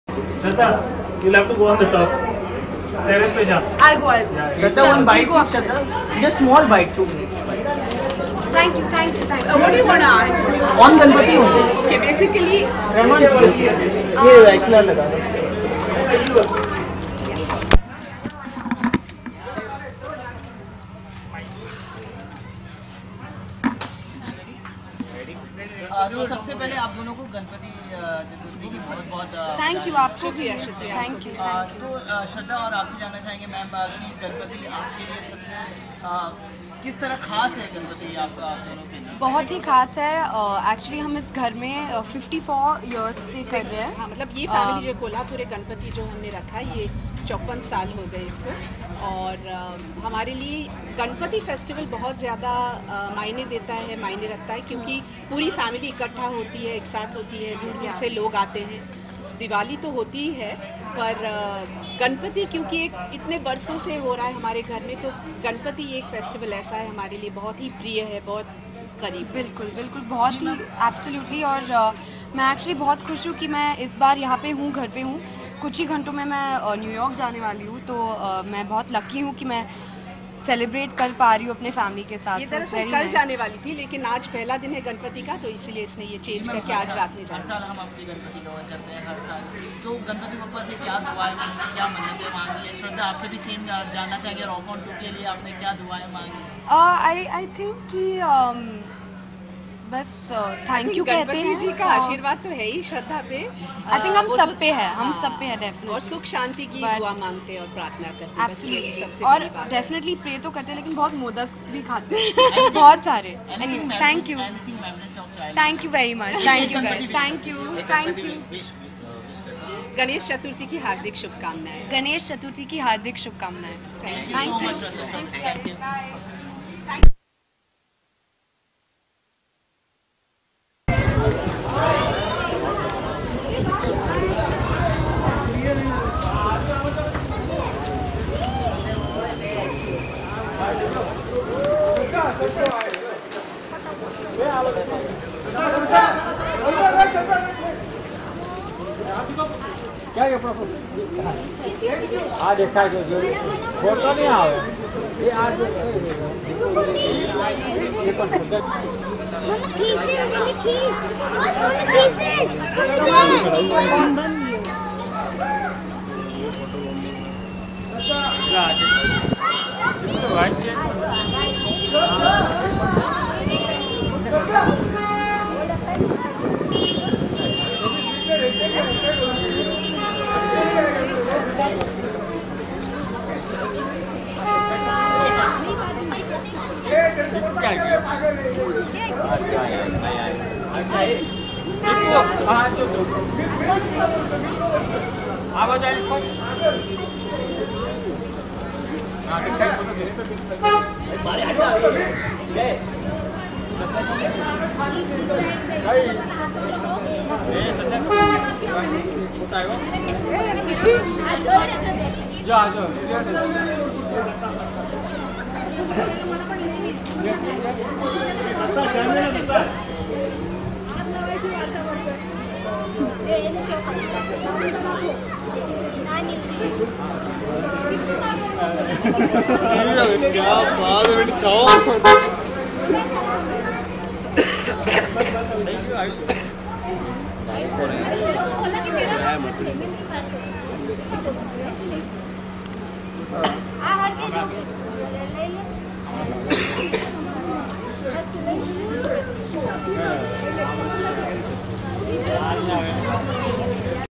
Shraddha Kapoor Celebrating Ganesh Chaturthi With Family l 2016
Shraddha Kapoor Celebrating Ganesh Chaturthi With Family